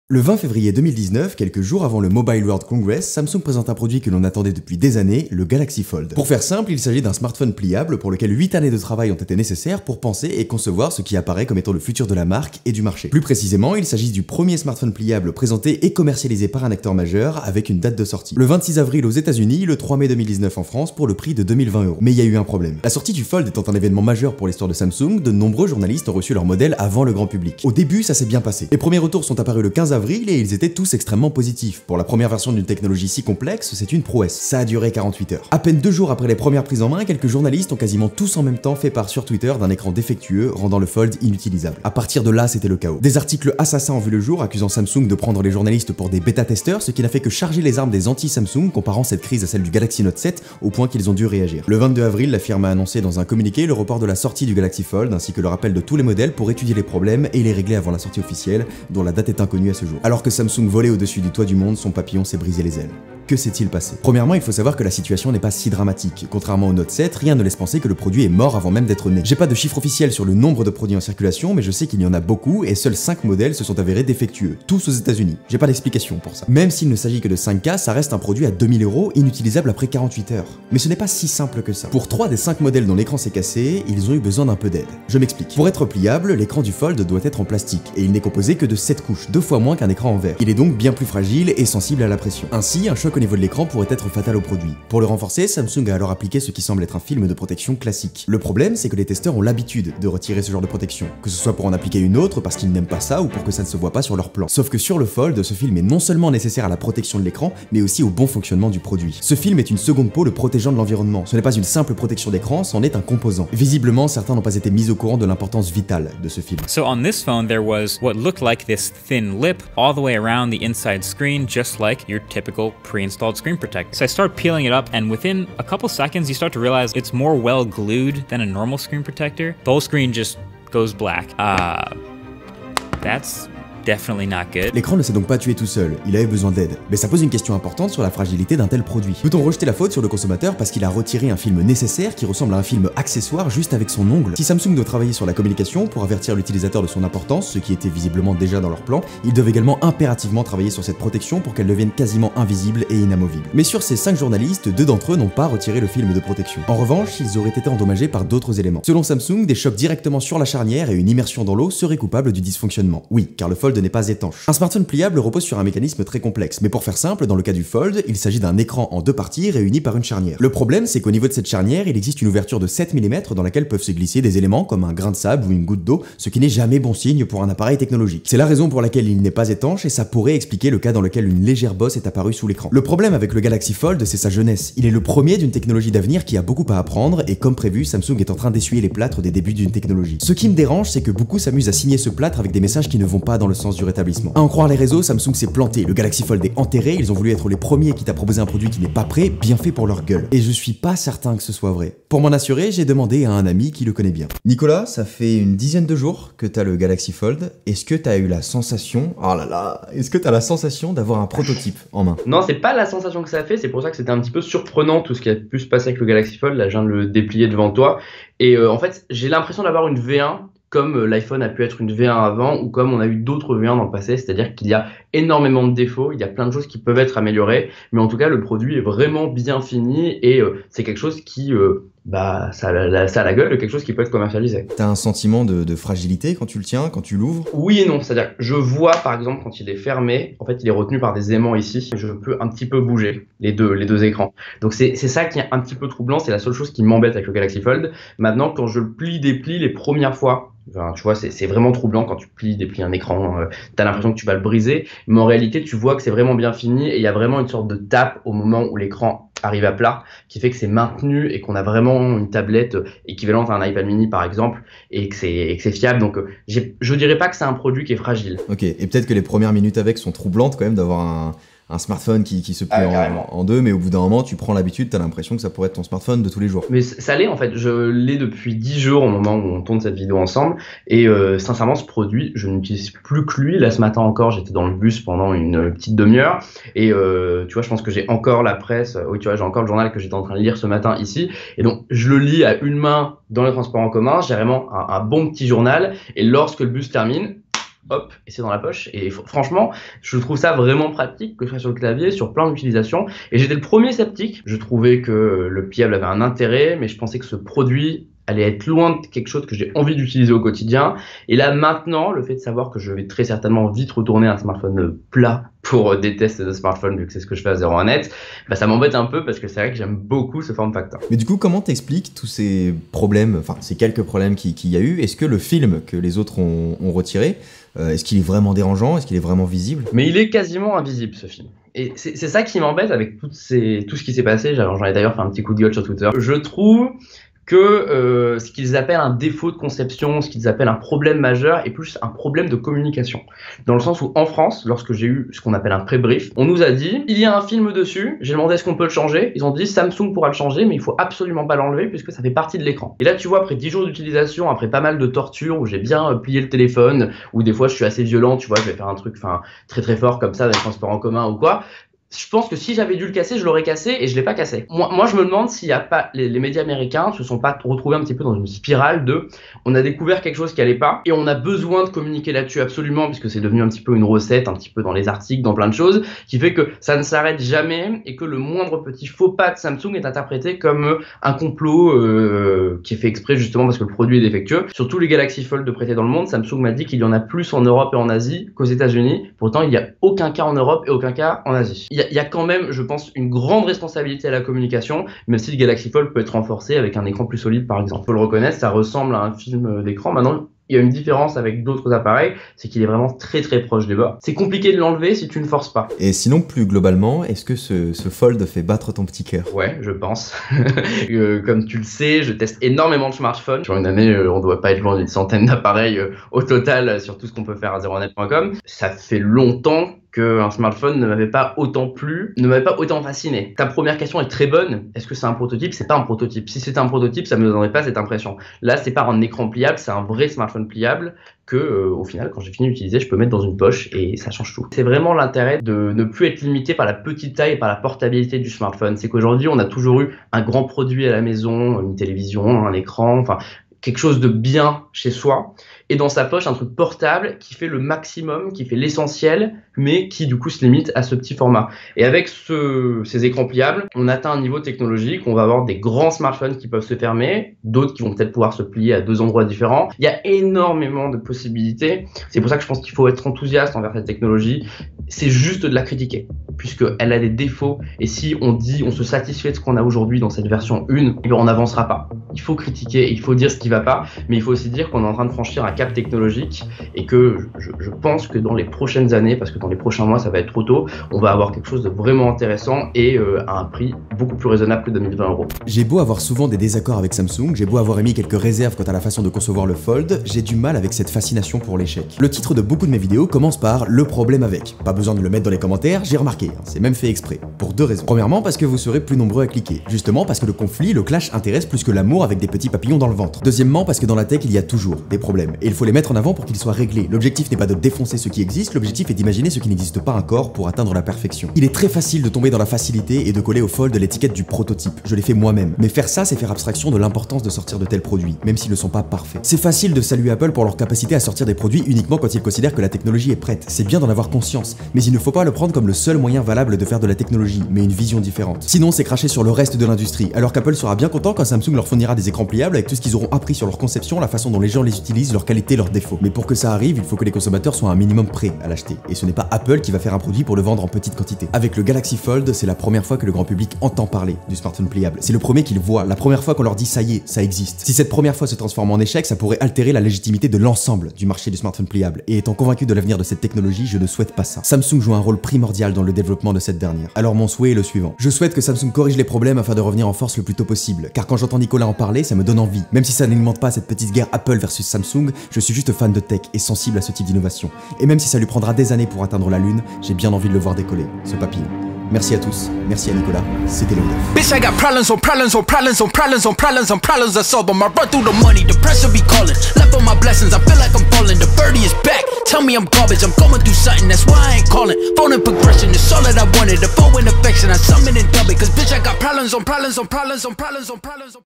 Le problème avec le Galaxy Fold !
Après des années d'attente, Samsung a enfin annoncé la sortie de son smartphone pliable : le Galaxy Fold ! Mais il y a eu un problème.

Les vidéos pour lesquelles je suis rémunéré affichent un message l'indiquant, au début de la vidéo, en bas à gauche.

Léo Duff.